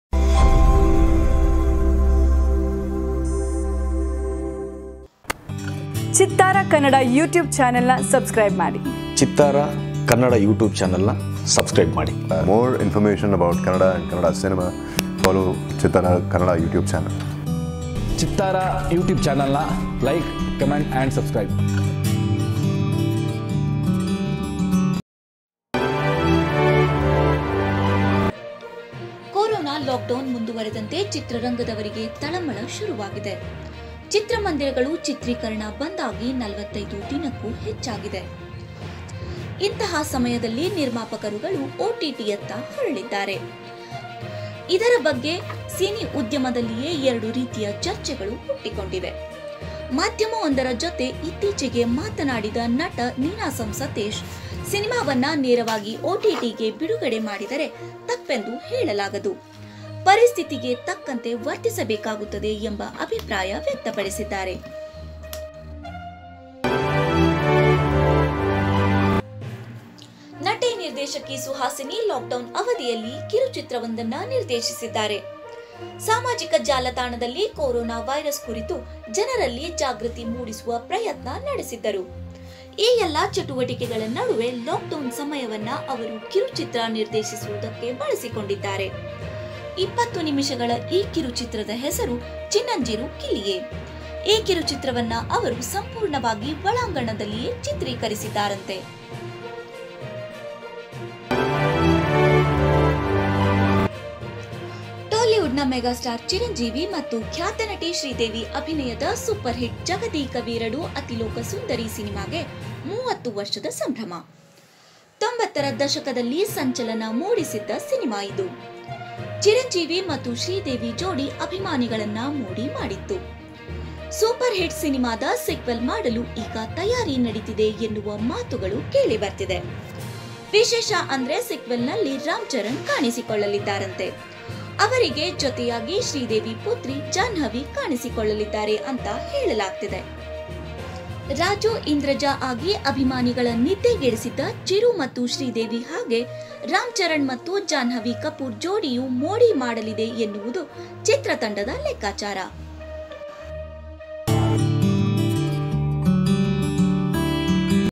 ರಾಮ್‌ಚರಣ್‌ಗೆ ಸಿಕ್ಬಿಟ್ಳು ಅತಿಲೋಕ ಸುಂದ್ರಿ! | Chittara Kannada | Cinema Express
ಓಟಿಟಿ ನಲ್ಲಿ ಸಿನಿಮಾ ರಿಲೀಸ್ ಮಾಡಿದರೆ ತಪ್ಪೆನ್ನಲಾಗದು: ಓಟಿಟಿ ಪರ ಸತೀಶ್ ನಿಲವು

ಕೊರೊನಾ ಲಾಕ್‌ಡೌನ್ ಮುಂದುವರೆದಂತೆ ಚಿತ್ರರಂಗದವರಿಗೆ ತಳಮಳ ಶುರುವಾಗಿದೆ. ಚಿತ್ರಮಂದಿರಗಳು, ಚಿತ್ರೀಕರಣ ಬಂದ್ ಆಗಿ 45 ಕ್ಕೂ ಮೇಲಾಯಿತು ಸಾಲತಂದು ಹಣ ಹೂಡಿ ಚಿತ್ರಬಿಡುಗಡೆಗೆ ಕಾಯುತ್ತಿದ್ದ ನಿರ್ಮಾಪಕರು ಲಾಕ್‌ಡೌನ್‌ನಿಂದ ಕಂಗಾಲಾಗಿದ್ದು ಚಿತ್ರಮಂದಿರಗಳು ಯಾವಾಗ ಪುನರರಾರಂಭ ಆಗುತ್ತವೆಯೆಂಬ ಭರವಸೆಯೂ ಇಲ್ಲ. ಇಂಥಹಾ ಸಮಯದಲ್ಲಿ ನಿರ್ಮಾಪಕರುಗಳು ಓಟಿಟಿಗಳತ್ತ ಹೊರಳಿದ್ದು, ಡಬ್ಬದಲ್ಲಿರುವ ಸಿನಿಮಾಗಳನ್ನು ನೆಟ್‌ಫ್ಲಿಕ್ಸ್, ಅಮೆಜಾನ್ ಪ್ರೈಂ, ವೂಟ್, ಡಿಸ್ನಿ ಪ್ಲಸ್ ರೀತಿಯ ಓಟಿಟಿಗಳಿಗೆ ಮಾರುವ ಯೋಚನೆ ಮಾಡುತ್ತಿದ್ದಾರೆ. ಆದರೆ ಈ ಬಗ್ಗೆ ಸಿನಿ ಉದ್ಯಮದಲ್ಲಿಯೇ ಎರಡು ರೀತಿಯ ಚರ್ಚೆಗಳು ಹುಟ್ಟಿವೆ. ಮಾಧ್ಯಮವೊಂದರ ಜೊತೆ ಇತ್ತೀಚೆಗೆ ಮಾತನಾಡಿದ ನಟ ಸತೀಶ್ ನೀನಾಸಂ, ಸಿನಿಮಾವನ್ನು ನೇರವಾಗಿ ಓಟಿಟಿ ಗೆ ಬಿಡುಗಡೆ ಮಾಡಿದರೆ ತಪ್ಪೆಂದು ಹೇಳಲಾಗದು, ಪರಿಸ್ಥಿತಿಗೆ ತಕ್ಕಂತೆ ವರ್ತಿಸಬೇಕಾಗುತ್ತದೆ ಎಂಬ ಅಭಿಪ್ರಾಯ ವ್ಯಕ್ತಪಡಿಸಿದ್ದಾರೆ.

ಚಿರಂಜೀವಿ-ಶ್ರೀದೇವಿ ಅಭಿನಯದ ಸೂಪರ್ ಹಿಟ್ ಸಿನಿಮಾದ ಸೀಕ್ವೆಲ್ ನಲ್ಲಿ ರಾಮ್ ಚರಣ್-ಜಾಹ್ನವಿ?

ಟಾಲಿವುಡ್ ಮೆಗಾ ಸ್ಟಾರ್ ಚಿರಂಜೀವಿ ಮತ್ತು ಖ್ಯಾತ ನಟಿ ಶ್ರೀದೇವಿ ಅಭಿನಯದ ಸೂಪರ್ ಹಿಟ್ 'ಜಗದೇಕ ವೀರುಡು ಅತಿಲೋಕ ಸುಂದರಿ' ಸಿನಿಮಾಗೆ 30 ವರ್ಷದ ಸಂಭ್ರಮ. 90ರ ದಶಕದಲ್ಲಿ ಸಂಚಲನ ಮೂಡಿಸಿದ್ದ ಸಿನಿಮಾವಿದು. ಚಿರಂಜೀವಿ ಮತ್ತು ಶ್ರೀದೇವಿ ಜೋಡಿ ಅಭಿಮಾನಿಗಳನ್ನು ಮೋಡಿ ಮಾಡಿತ್ತು. ಸೂಪರ್ ಹಿಟ್ ಸಿನಿಮಾದ ಸೀಕ್ವೆಲ್ ಮಾಡಲು ತಯಾರಿ ನಡೆಯುತ್ತಿದೆ ಎನ್ನುವ ಮಾತು ಕೇಳಿ ಬರುತ್ತಿದೆ.ವಿಶೇಷ ಅಂದರೆ ಸೀಕ್ವೆಲ್ ನಲ್ಲಿ ರಾಮ್ ಚರಣ್ ಅವರೆ ಕಾಣಿಸಿಕೊಳ್ಳಲಿದ್ದಾರೆ ಎಂದು ಹೇಳಲಾಗುತ್ತಿದೆ. ರಾಮ್ ಚರಣ್ ಗೆ ನಾಯಕಿಯಾಗಿ ಶ್ರೀದೇವಿ ಪುತ್ರಿ ಜಾಹ್ನವಿ ಕಪೂರ್ ಕಾಣಿಸಿಕೊಳ್ಳಲಿದ್ದಾರೆ ಎಂದು ಹೇಳಲಾಗುತ್ತಿದೆ.
ರಾಜು-ಇಂದ್ರಜಾ ಆಗಿ ಅಭಿಮಾನಿಗಳ ನಿದ್ದೆಗೆಡಿಸಿದ್ದ ಚಿರು ಮತ್ತು ಶ್ರೀದೇವಿ ಹಾಗೆ ರಾಮ್ ಚರಣ್ ಮತ್ತು ಜಾಹ್ನವಿ ಕಪೂರ್ ಜೋಡಿಯೂ ಮೋಡಿ ಮಾಡಲಿದೆ ಎನ್ನುವುದು ಚಿತ್ರತಂಡದ ಲೆಕ್ಕಾಚಾರ.

ಲಾಕ್‌ಡೌನ್‌ನಲ್ಲಿಯೇ ಕಿರುಚಿತ್ರ ನಿರ್ದೇಶಿಸಿದ ಸುಹಾಸಿನಿ ಮಣಿರತ್ನಂ

ನಟಿ, ನಿರ್ದೇಶಕಿ ಸುಹಾಸಿನಿ ಲಾಕ್ ಡೌನ್ ಅವಧಿಯಲ್ಲಿಯೇ ಕಿರುಚಿತ್ರವೊಂದನ್ನು ನಿರ್ದೇಶಿಸಿದ್ದಾರೆ. ಕೊರೊನಾ ವೈರಸ್ ಕುರಿತು ಜನರಲ್ಲಿ ಜಾಗೃತಿ ಮೂಡಿಸುವ ಪ್ರಯತ್ನ ನಡೆಸುತ್ತಿದ್ದರು. ಈ ಎಲ್ಲ ಚಟುವಟಿಕೆಗಳ ನಡುವೆಯೇ ಲಾಕ್ ಡೌನ್ ಸಮಯವನ್ನು ಅವರು ಕಿರುಚಿತ್ರ ನಿರ್ದೇಶನಕ್ಕೆ ಬಳಸಿಕೊಂಡಿದ್ದಾರೆ. 20 ನಿಮಿಷಗಳ ಈ ಕಿರುಚಿತ್ರದ ಹೆಸರು 'ಚಿನ್ನಂಜಿರು ಕಿಲಿಯೆ'. ಈ ಕಿರುಚಿತ್ರವನ್ನು ಅವರು ಸಂಪೂರ್ಣವಾಗಿ ಒಳಾಂಗಣದಲ್ಲಿಯೇ ಚಿತ್ರೀಕರಿಸಿದ್ದಾರಂತೆ.

Hello and welcome to CHITTARA KANNADA Entertainment YouTube Channel.